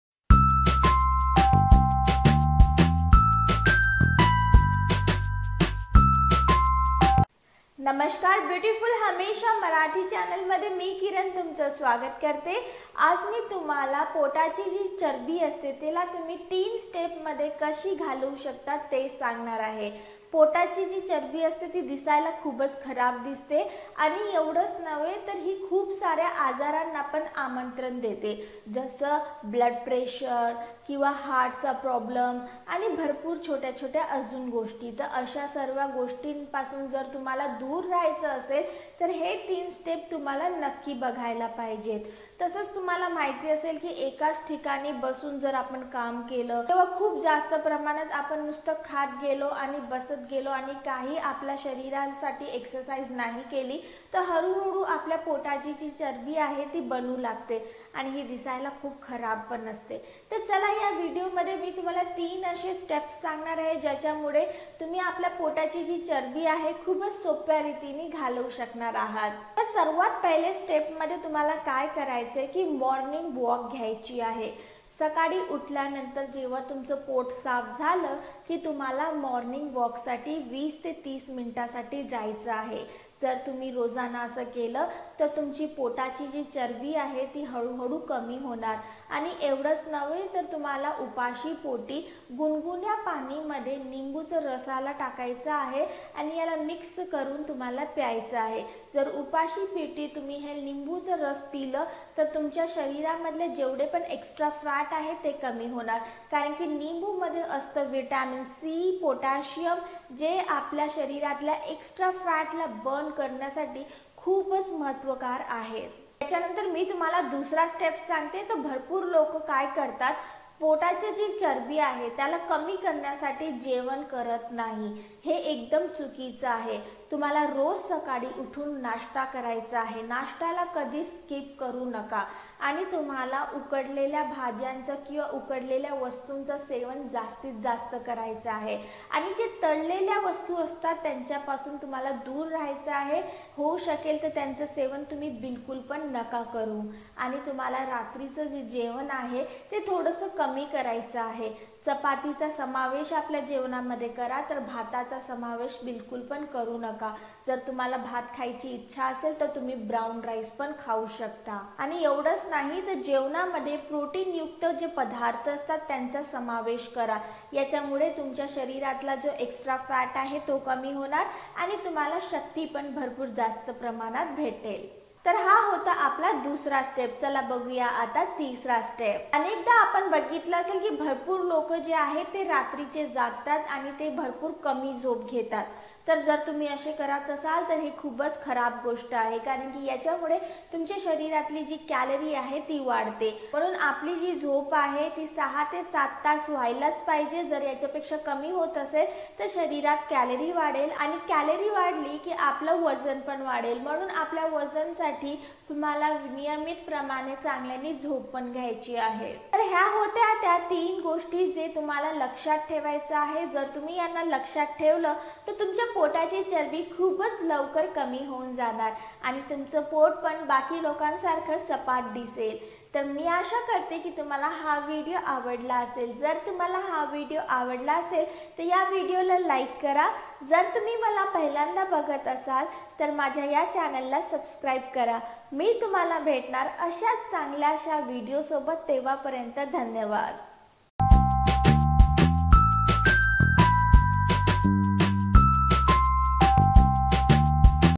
Belly Fat पोटाची चरबी कमी करा फक्त 3 स्टेप्स च्या मदतीने | 3 Steps to Reduce Belly Fat easily
पोटाची चरबी कमी करा फक्त 3 स्टेप्स च्या मदतीने | 3 Steps to Reduce Belly Fat easily -
In this Video, I will tell you 3 simple Steps to Reduce Belly Fat. Wajan Kmi Karnyache Steps Janun Ghenyasathi Ha Video Paha. Video Marathi Madhe Aahe

स्टेप 1 - जास्तीत जास्त चाला आणि सकाळी लिंबू पाणी प्या
स्टेप 2 - प्रोटीन युक्त जेवण करा
स्टेप 3 - चांगली झोप घ्या

Step 1 - Try to Walk More & Drink Lemon Water in Morning
Step 2 - Eat protein-rich food
Step 3 - Take good sleep

Ajun Upay Janun Ghenyasathi Ha Video Paha.